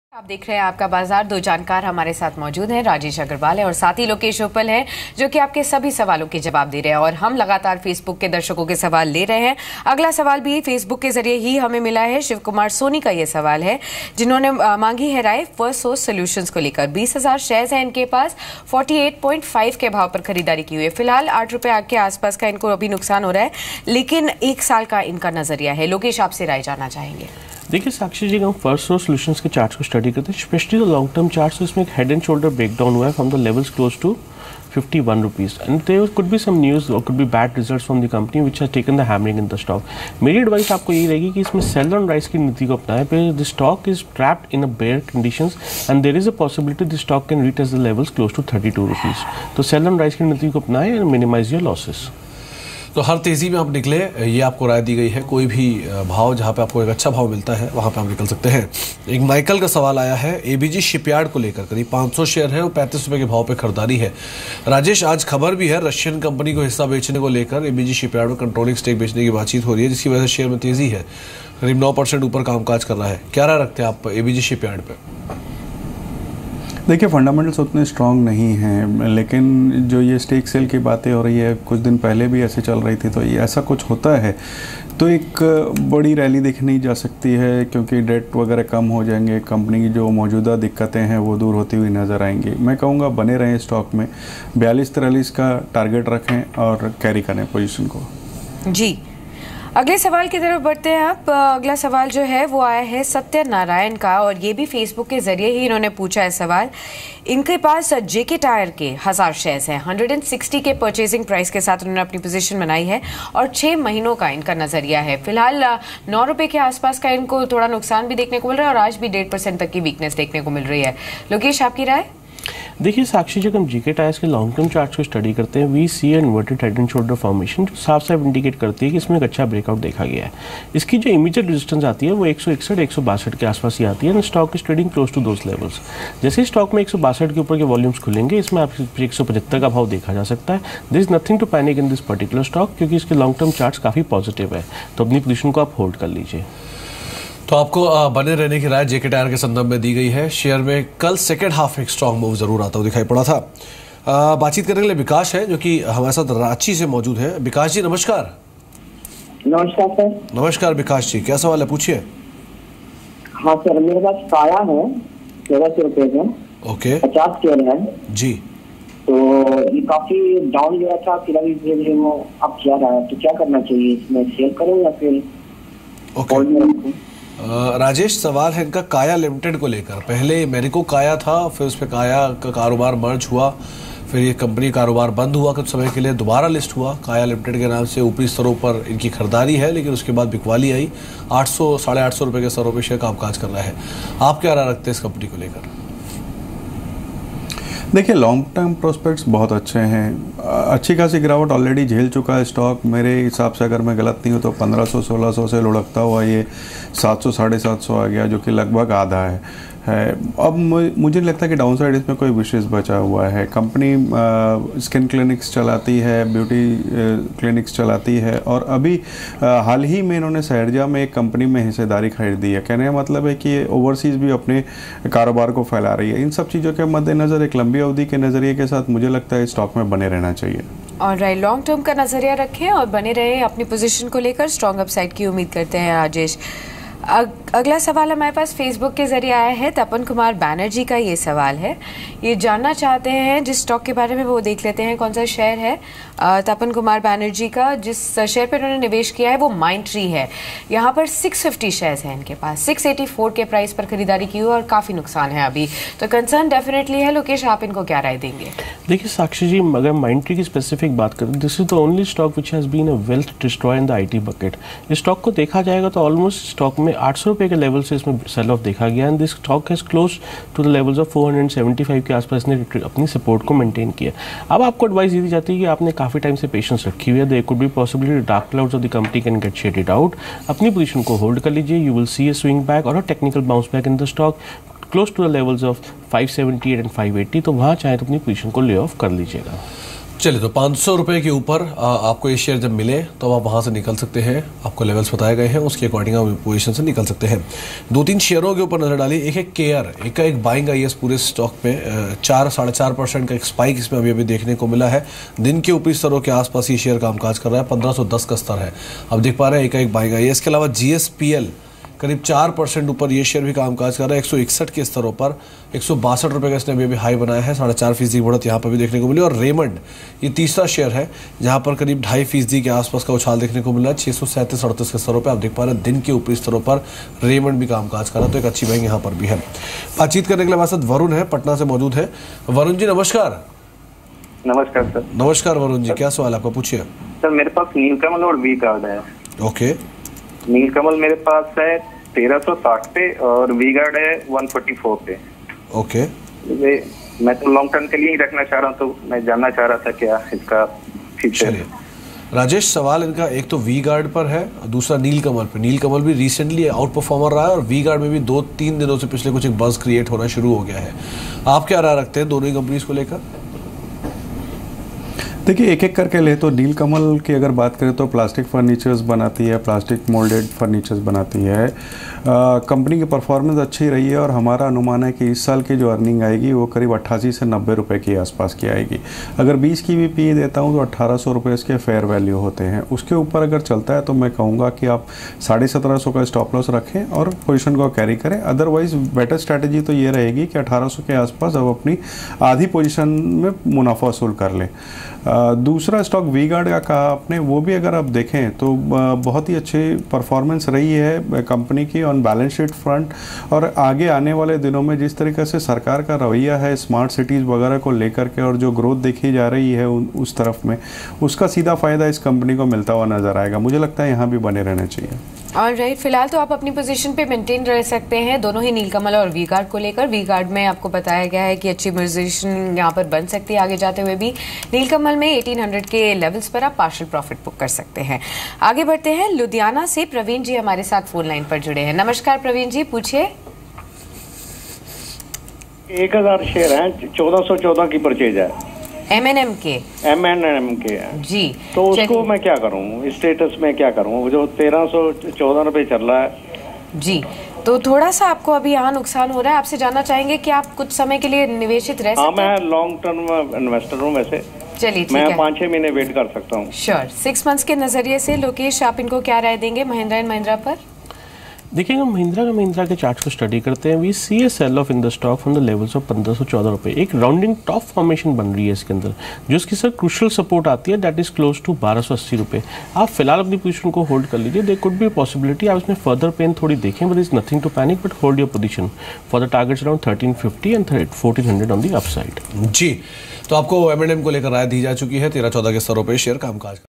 Experts outlook and suggestion on the stocks of Firstsource, ABG Shipyard, JK Tyre etc
Experts outlook and suggestion on the stocks of Firstsource, ABG Shipyard, JK Tyre etc. Watch this special segment and get to know more here.